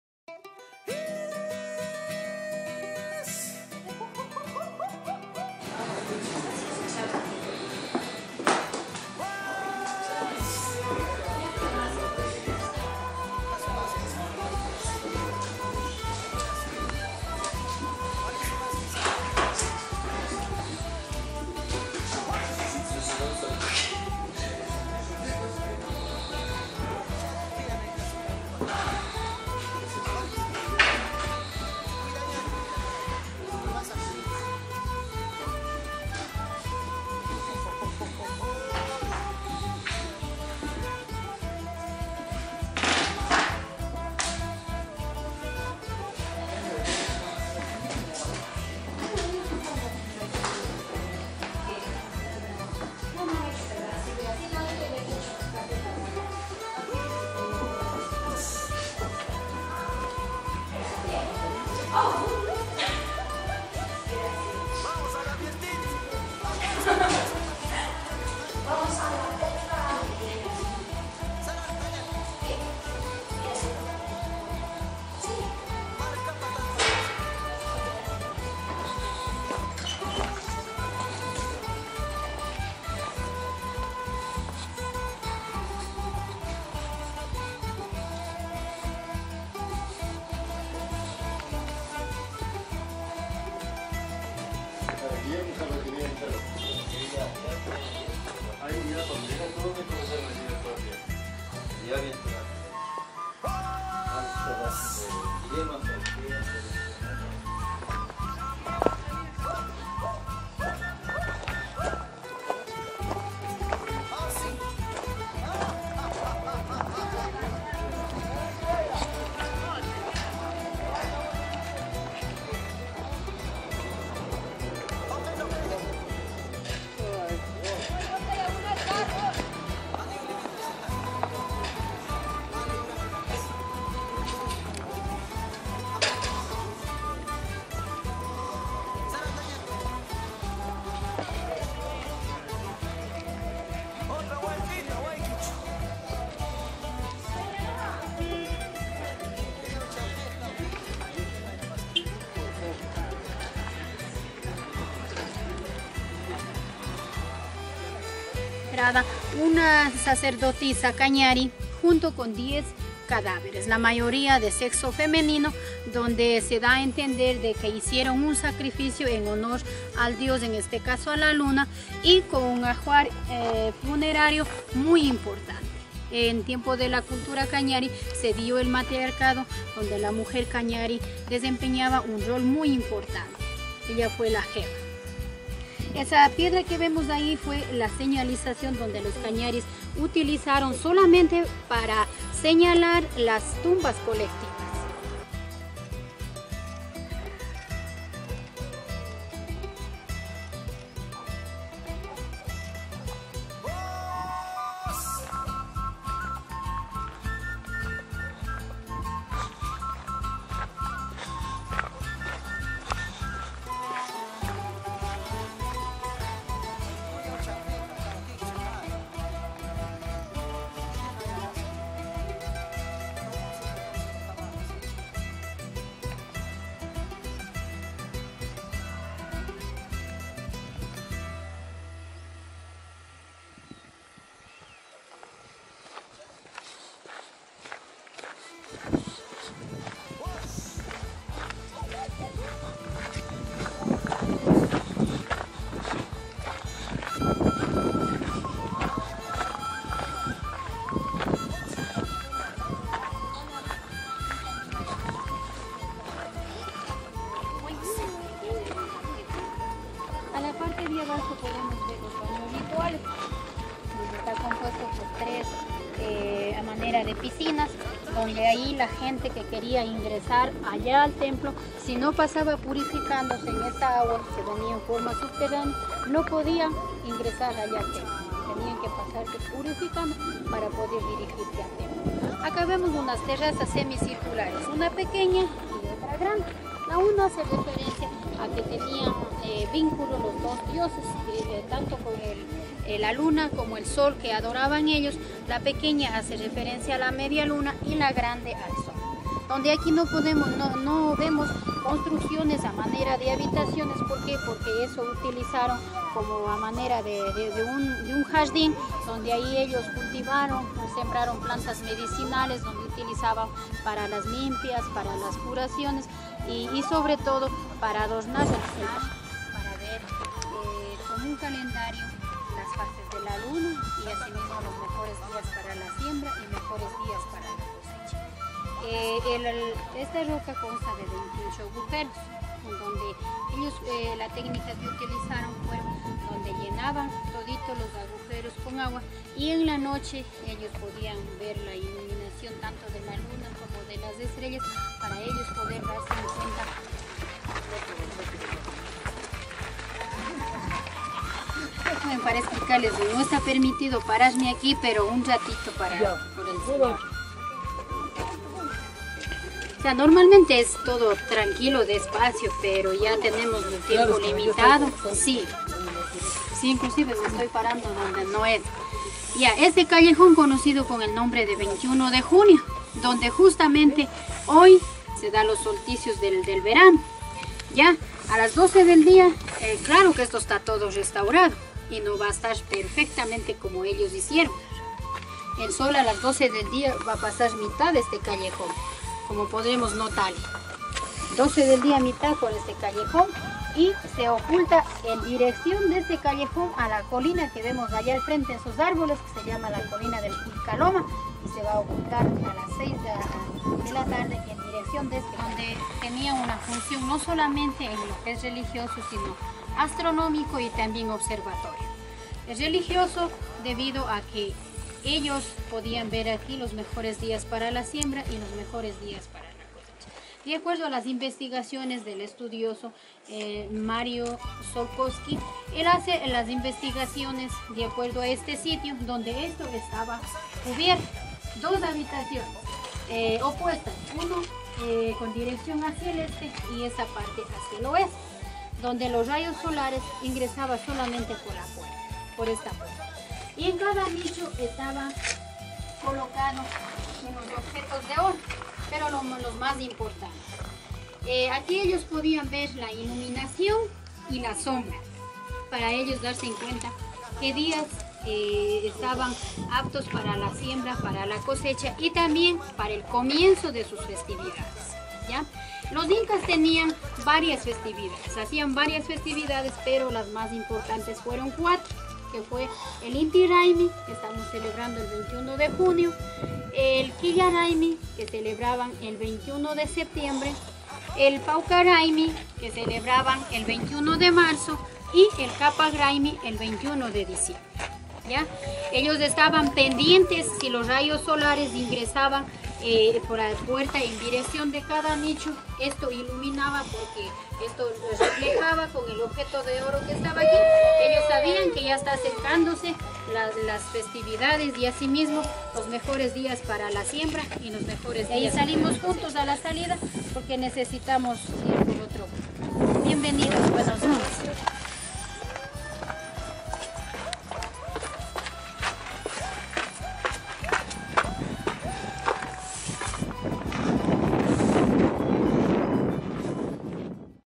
PROTOCOLO DE VISITA TURISTICA EN INGAPIRCA - ECUADOR
Les compartimos un video que grabamos en INGAPIRCA (Ecuador) en donde se puede apreciar todas las facilidades que se brindan a los visitantes (baños incluidos), Centro de Interpretación, Conectividad (WiFi y Correo), Oficina de Información, Tópico, Multimedia, Salas de Conferencias, Áreas Sociales, Anfiteatro y el Servicio de Guía incluido en el Ticket de ingreso.
Los profesionales Guías están debidamente capacitados, uniformados, acreditados y siguen un Guion de Visita (sugerido por el Parque) y un PROTOCOLO DE VISITA (obligado por el Parque) de esta manera se controla el recorrido, los tiempos, las zonas críticas, el periplo, el flujo de visitantes, todo procurando los propósitos arriba enunciados de satisfacción y protección…
Poner atención a los minutos 05:54 y 08:29 donde se ve como se cumple el Protocolo de Visita por parte de la señorita Guía!!!
Atentamente,
Lic. Leylys Gutiérrez